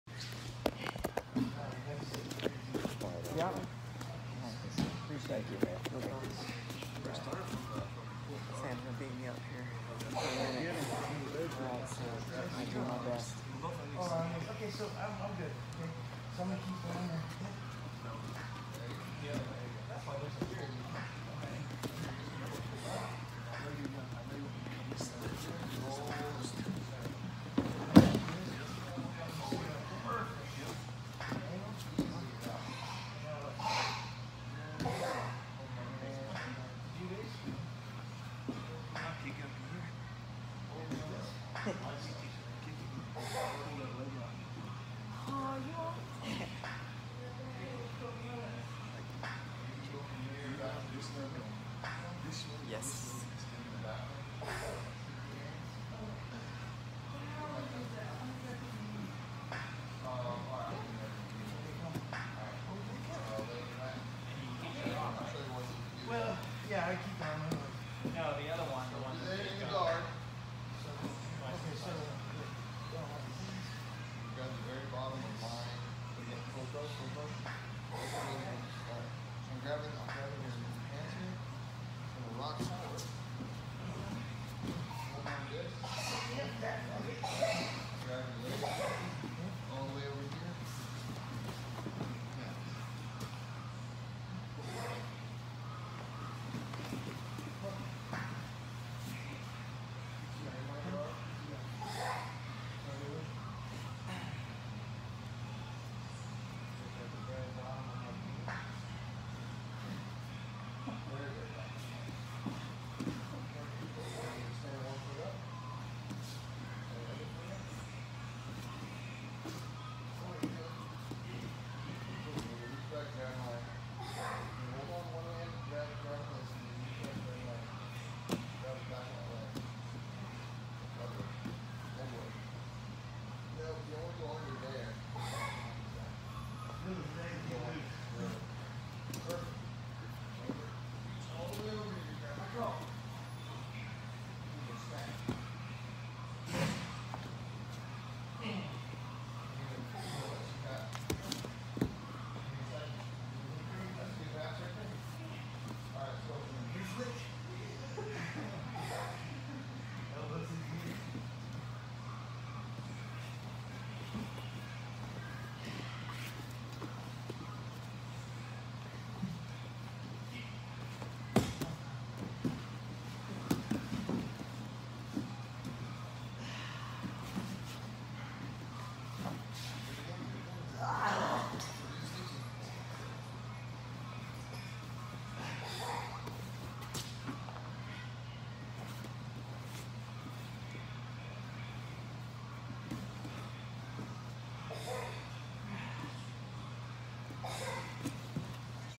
How to improve your jiu-jitsu student’s fundamentals the Helio Gracie way
This is just a few minutes of rolling with a student as an example of how to challenge them and test their technique without intensity or the need for victory.

Roll with your students at their pace. Put yourself in the positions that should trigger the different techniques and then praise every detail. Praise everything! Notice that when little mistakes happen, I don’t call attention to it or tell them it’s wrong - I fix their mistake for them and then say “perfect!”

The “perfect adjustment” is the undercover method for correcting mistakes absent of negativity.